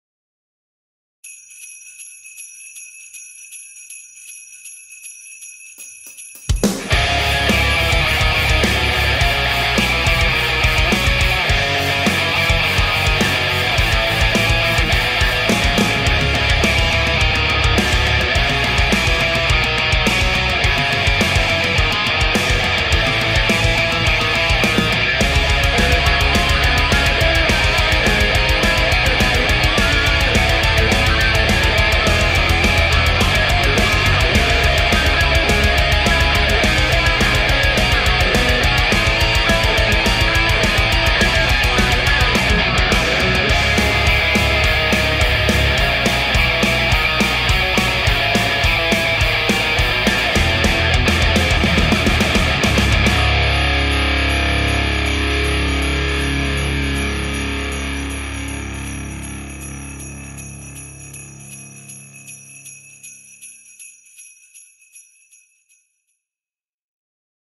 swola115 - Ola Riff challenge
I used this for my entry:

-Squier Showmaster
-Hiwatt Maxwatt G 100
-Digitech RP 255 Multieffect and Wah
 Boss GE-7 EQ        (in effect loop)
-Nux Solid Studio Impulse response pedal    (Amp Line out)
-Behringer Ultra-DI box
-Behringer UMC 204HD audio interface